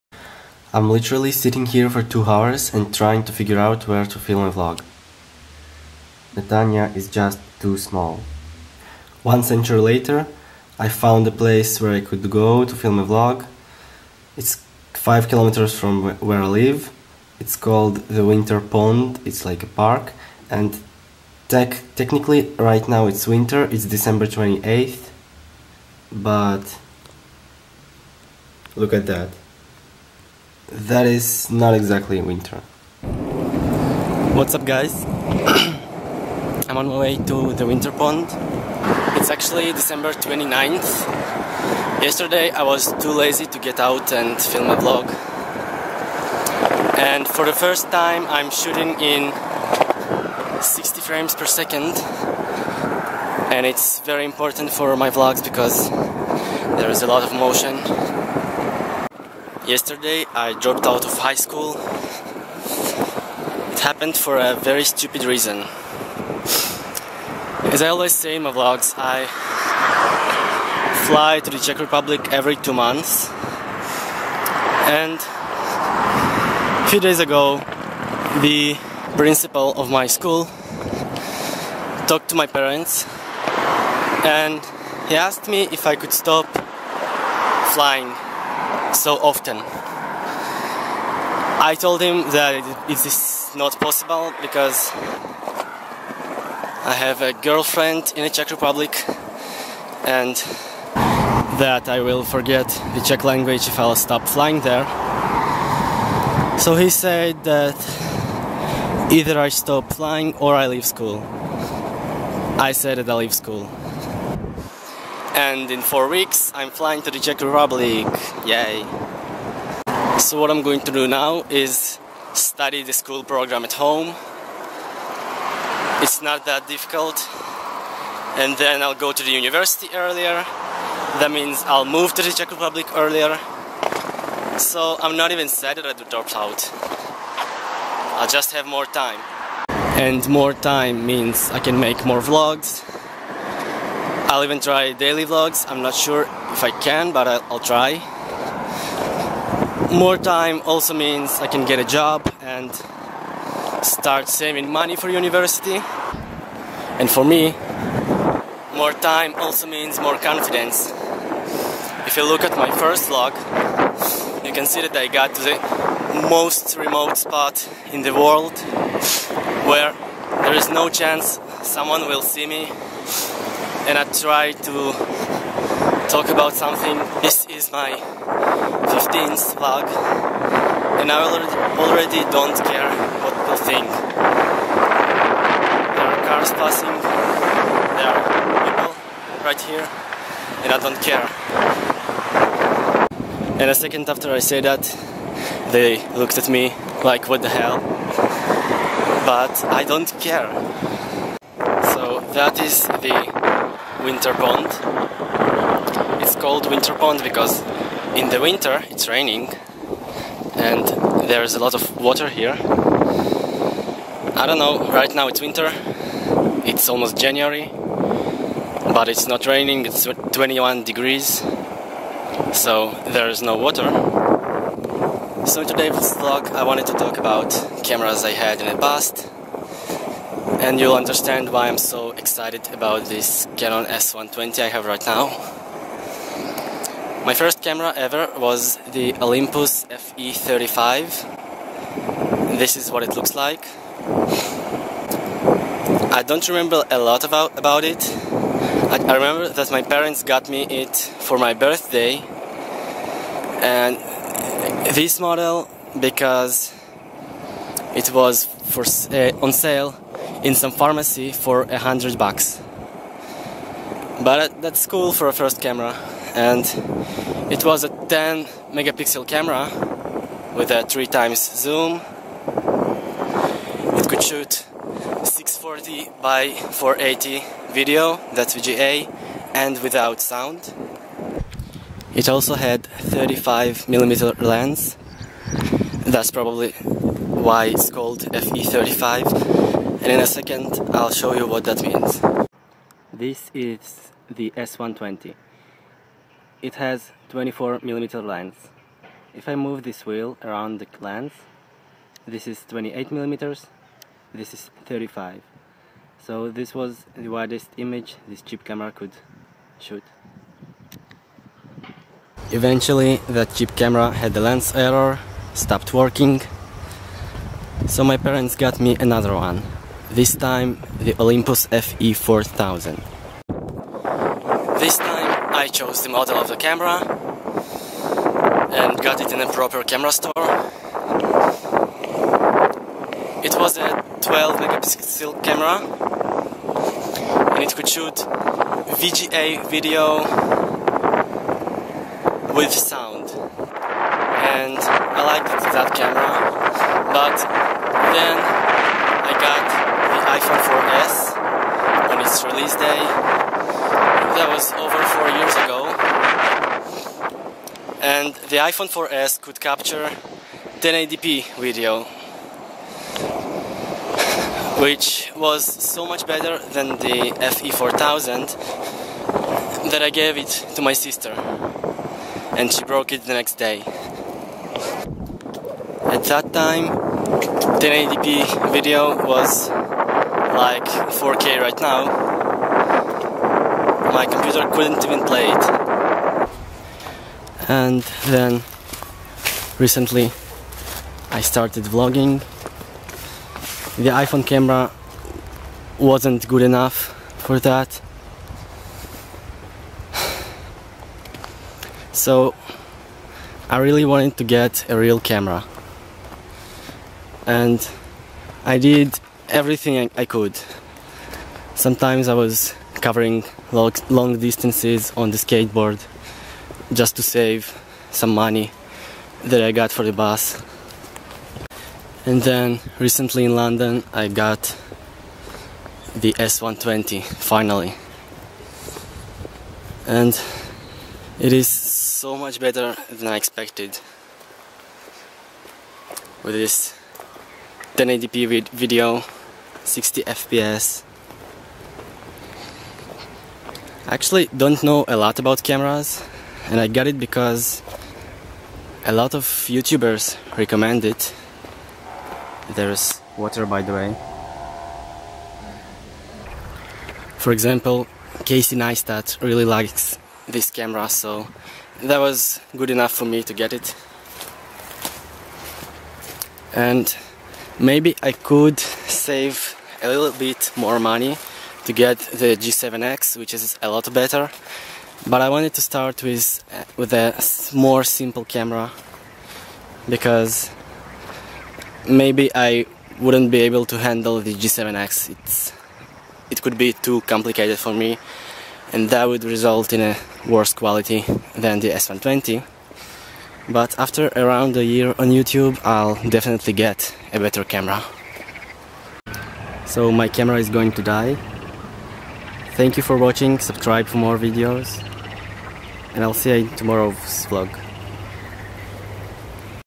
Cameras I had in the past
In this vlog I talk about cameras I had in the past and some stuff that has happened to me recently... If you liked it, subscribe for more videos :) also, I hope iMovie actually kept it in 60fps...
*update* it didn't. I'll have to film at 30fps